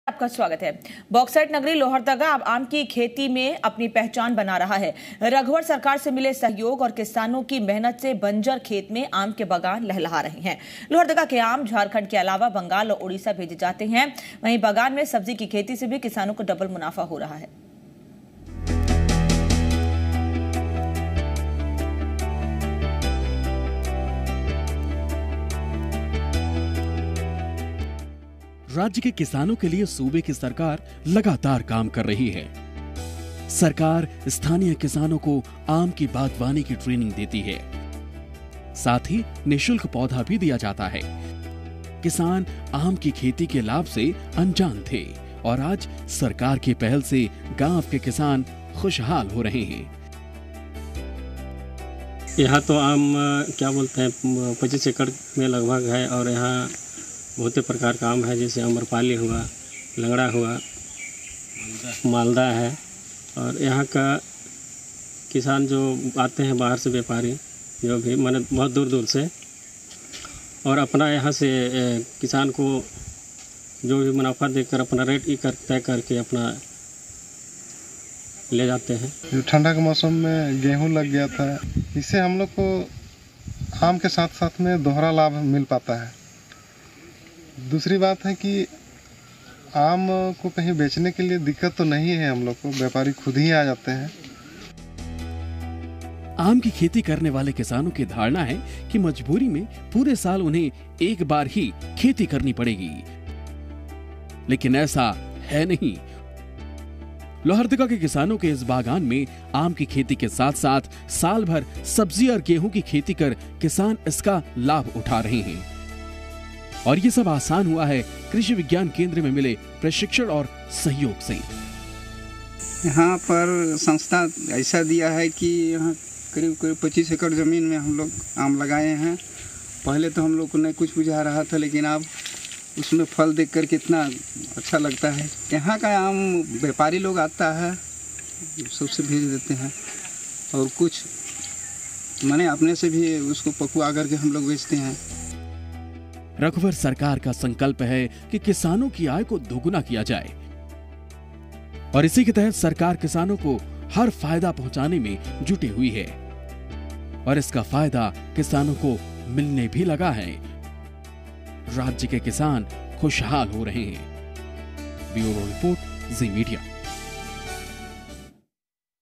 उन्नत झारखंड: आम की खेती में पहचान बना रहा लोहरदगा ।। Unnat Jharkhand
बॉक्साइट नगरी लोहरदगा अब आम की खेती में अपनी पहचान बना रहा है...रघुवर सरकार से मिले सहयोग और किसानों की मेहनत से बंजर खेत में आम के बगान लहलहा रहे हैं...लोहरदगा के आम झारखंड के अलावा बंगाल और ओडिशा भेजे जाते हैं...वहीं बगान में सब्जी की खेती से भी किसानों को डबल मुनाफा हो रहा है..।